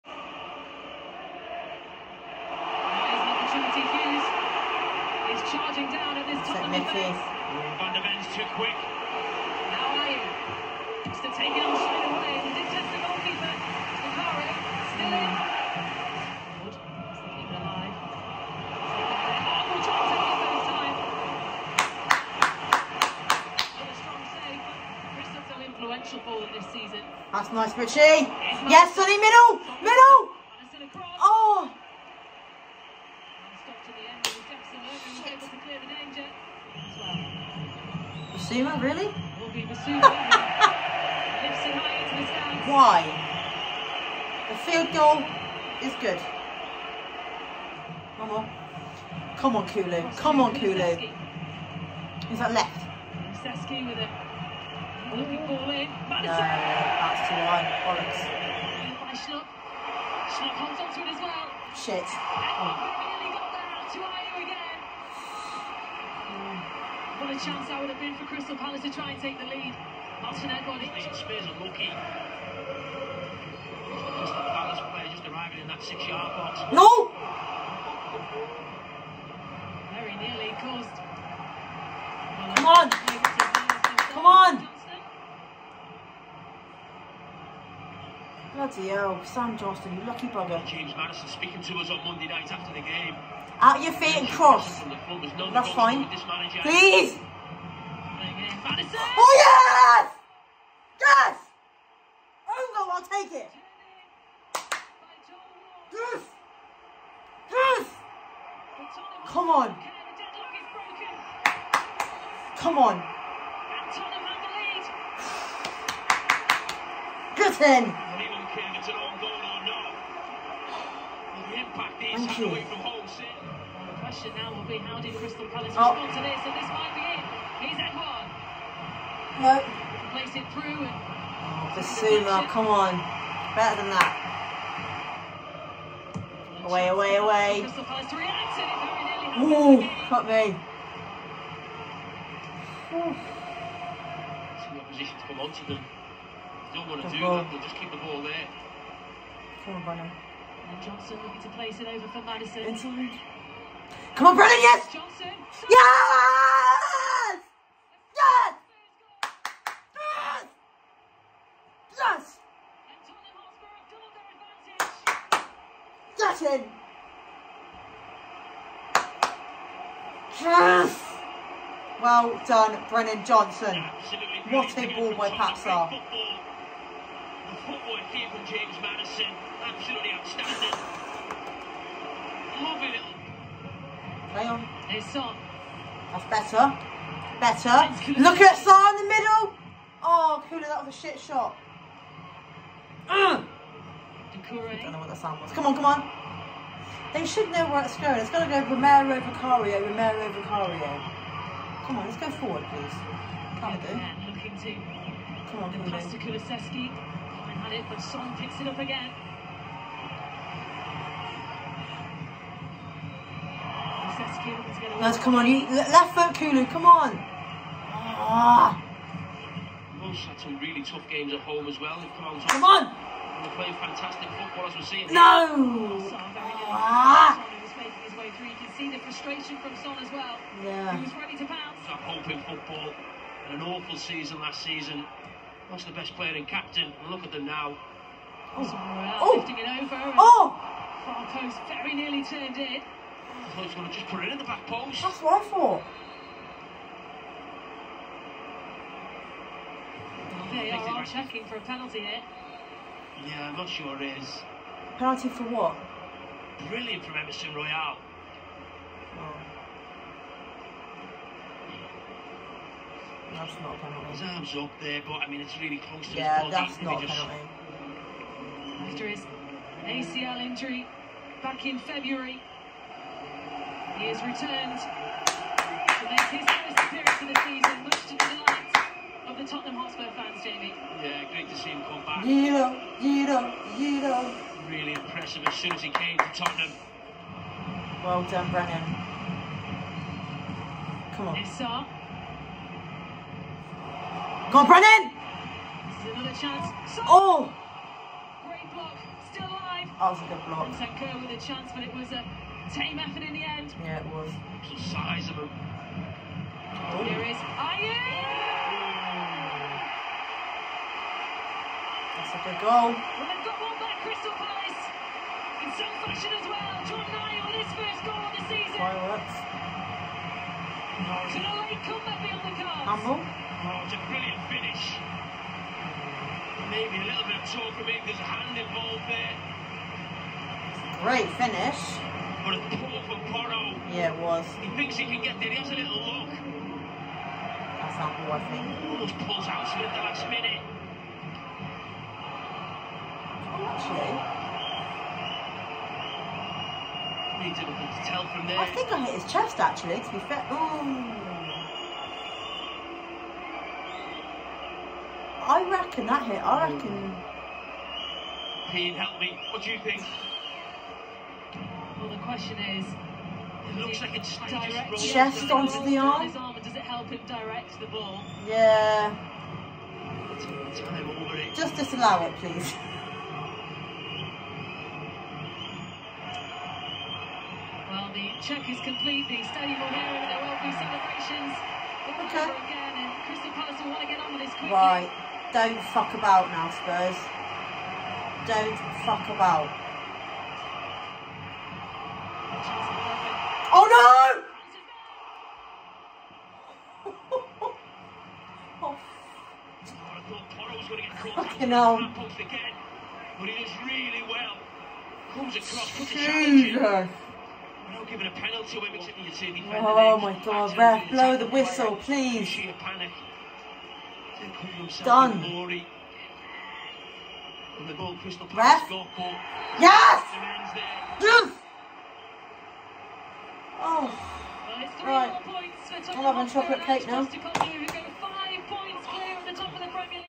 CRYSTAL PALACE 1-2 TOTTENHAM HOTSPUR - PREMIER LEAGUE MATCH DAY 10 - LIVE REACTION
Apologies I haven’t done a video in a while. I didn’t react to Luton as I no longer have TNT Sports, and I didn’t Vlog the Fulham game as I was celebrating my birthday.

I will only react to away games that are broadcasted on sky sports and of course to home games.

A great start to the season. Top of the the league on my birthday, 5 points clear. A long way to go though. COYS 🤍